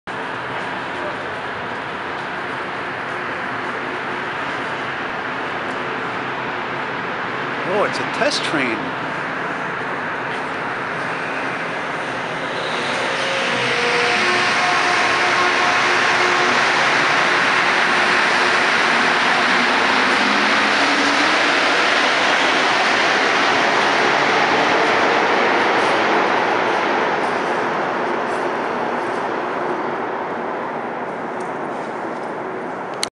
MONTREAL MR-73 METRO TEST TRAIN ACTION
SEEN AT SNOWDON METRO STATION ON THE ORANGE LINE 2.